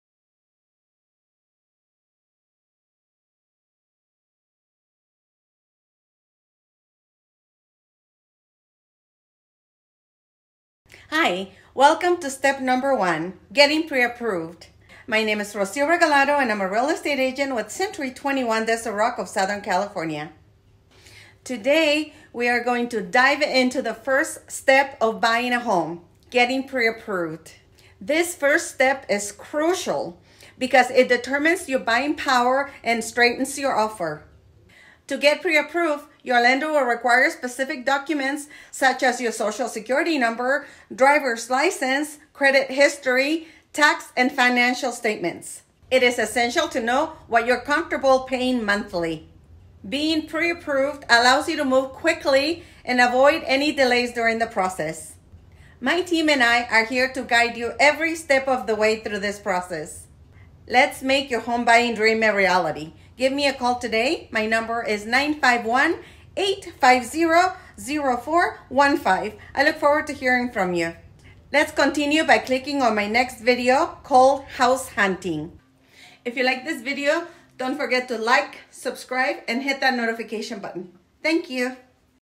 Home Buying Process Step 1: Getting Pre Approved
In Step 1 of the Home-Buying Process, I walk you through getting pre-approved.

Rocio Regalado
DRE: 02045224